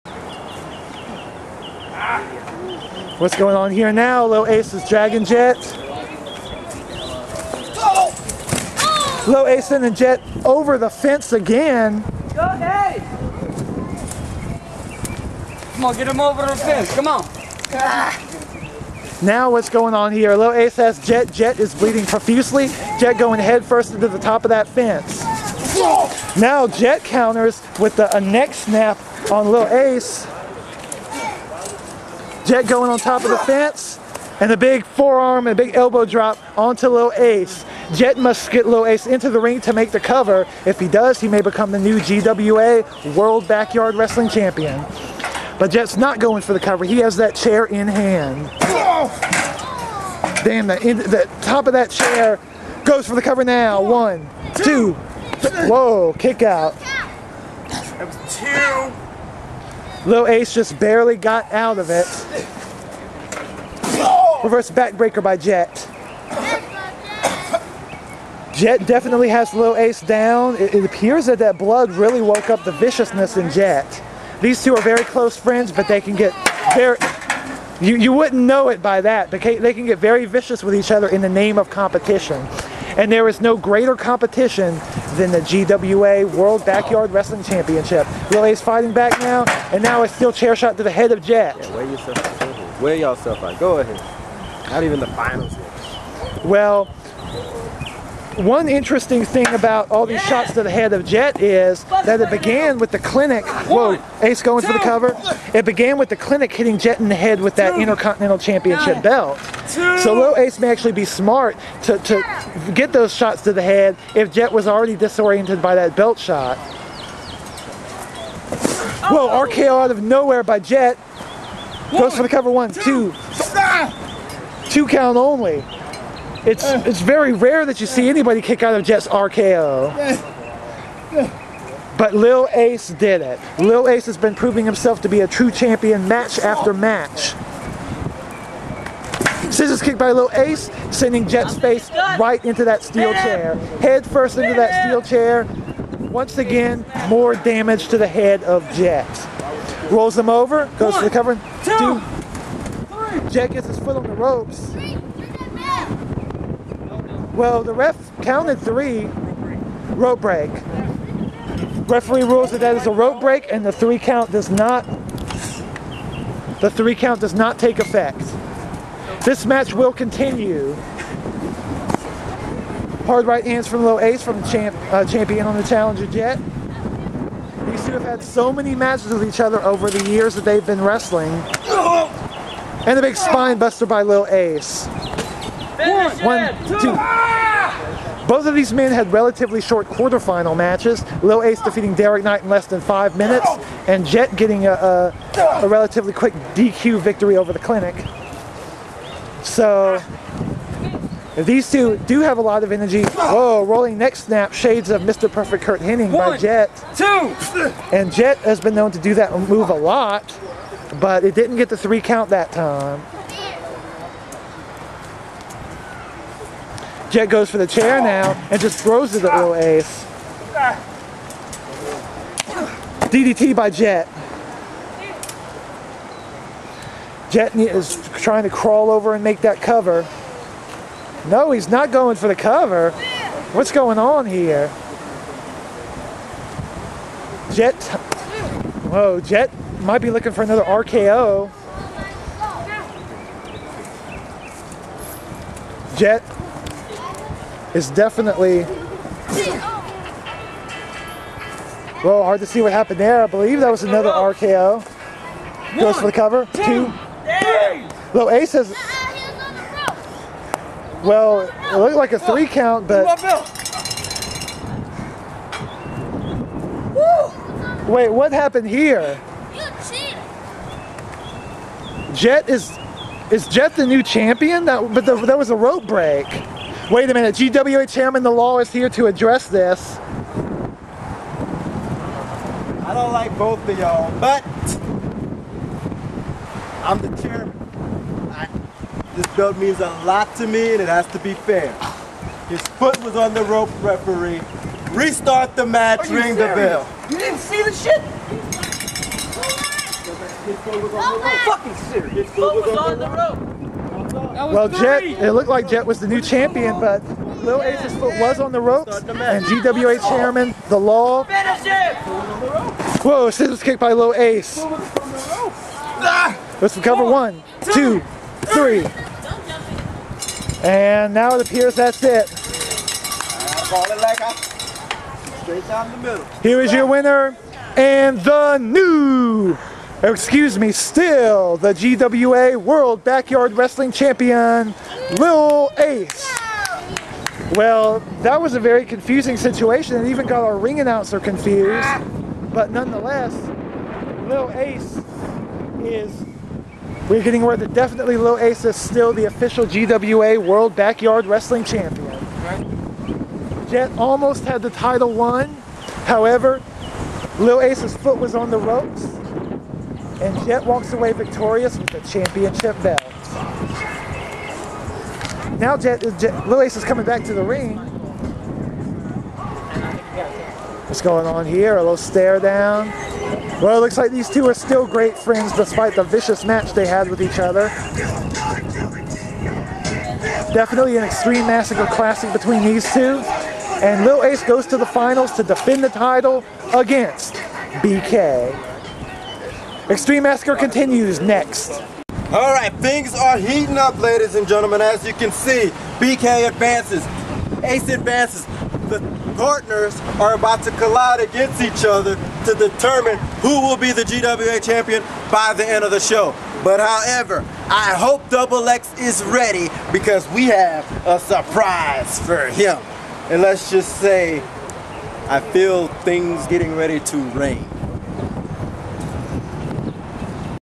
Jett vs. Lil Ace 4/2/10 (Part 2 of 2)
Lil Ace defends the GWA World Backyard Wrestling Championship against Jett in an Xtreme Massacre tournament semifinal at UWA Xtreme Massacre on April 2, 2010!